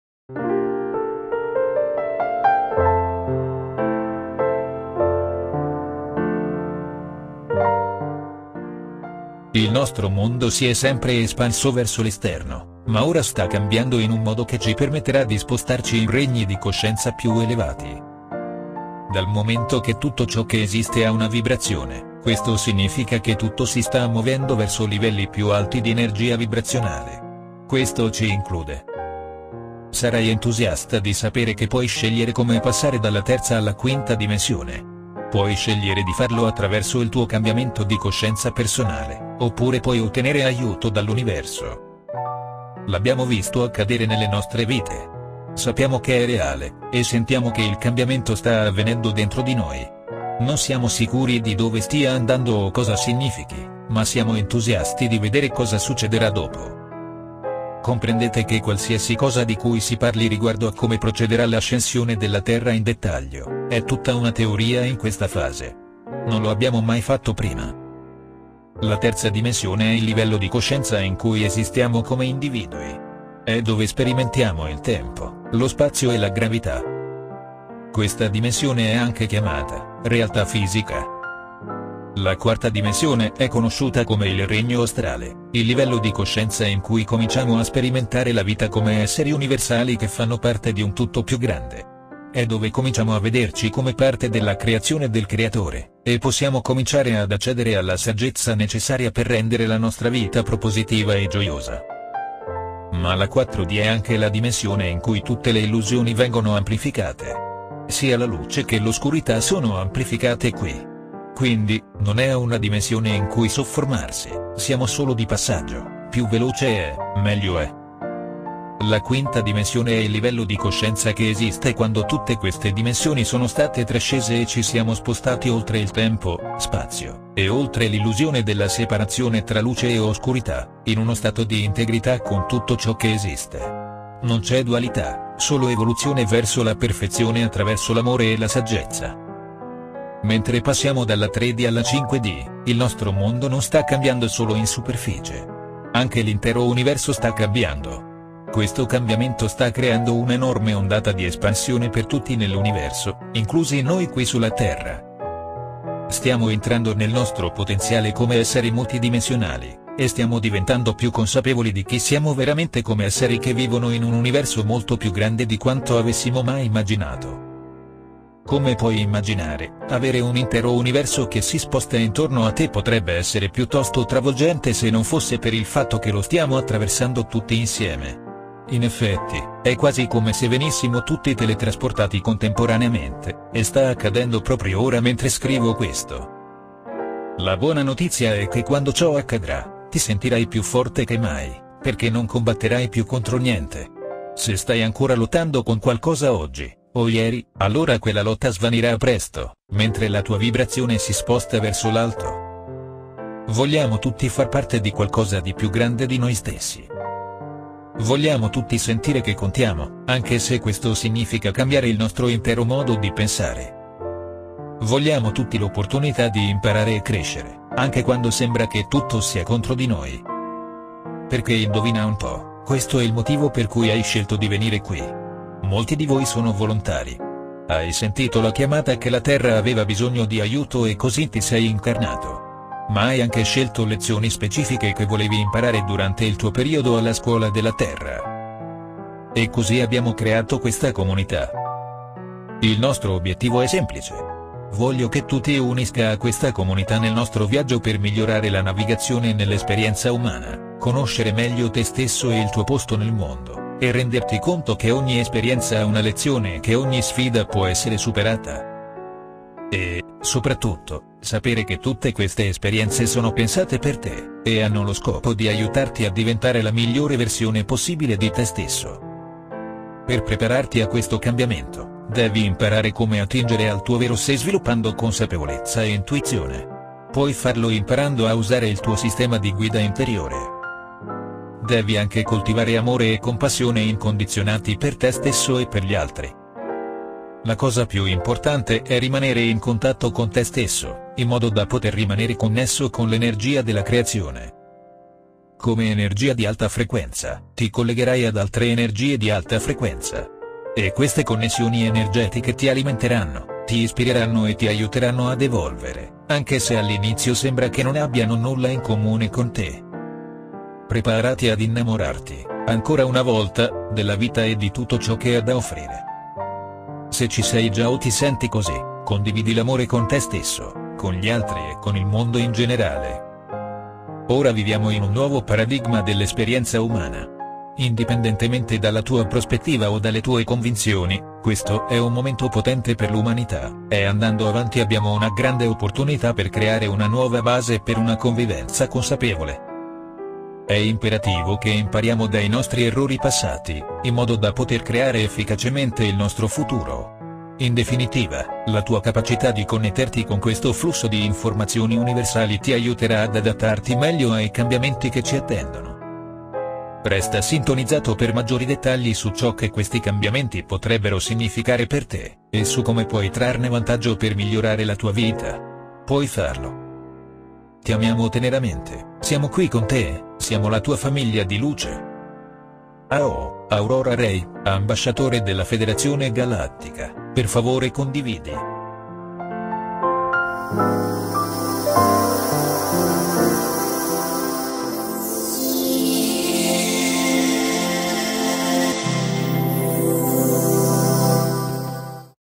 DA 3D A 5D – INCONTRA IL TUO FUTURO
Il nostro mondo si è sempre espanso verso l'esterno, ma ora sta cambiando in un modo che ci permetterà di spostarci in regni di coscienza più elevati.
::::::::::::::::::::::::::::Omissis:::::::::::::::::::::::::::::